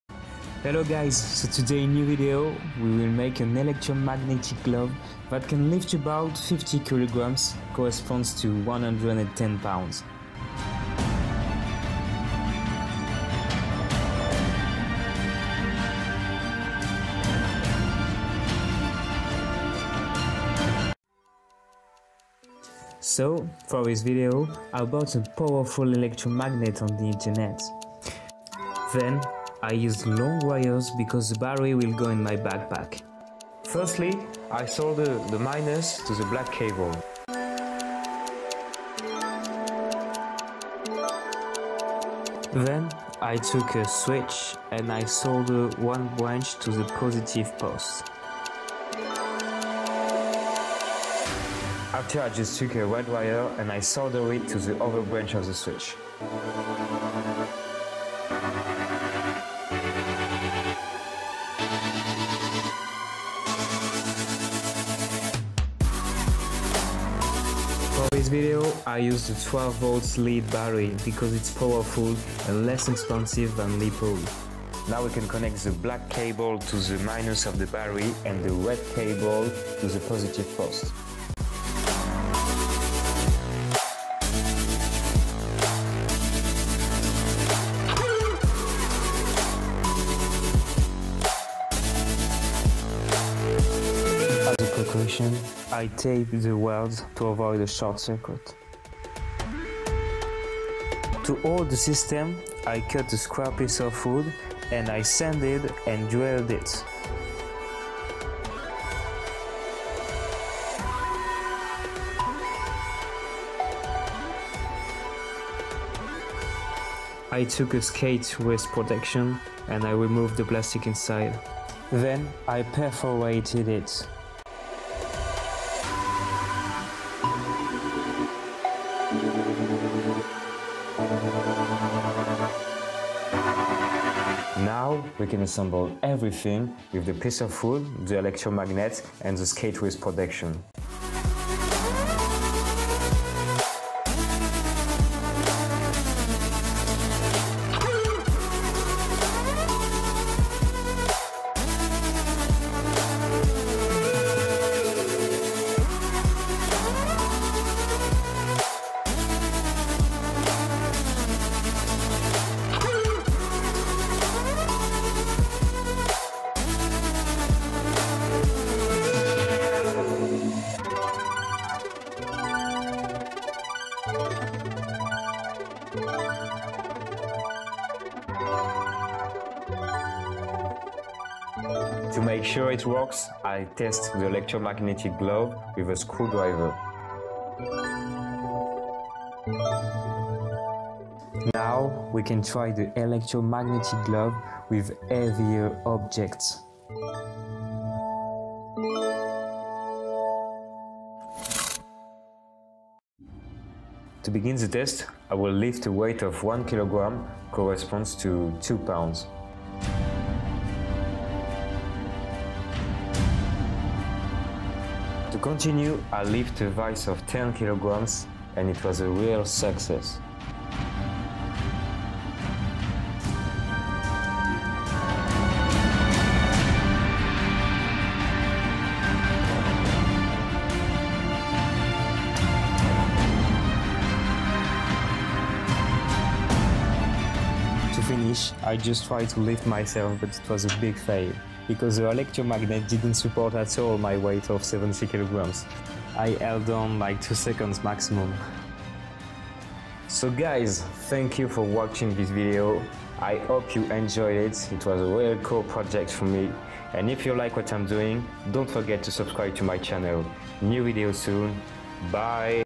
How to Make an Electromagnetic Glove !
Hi guys, today new video and I'm making an electromagnetic glove which can lift about 110lbs.
See you soon, Melchior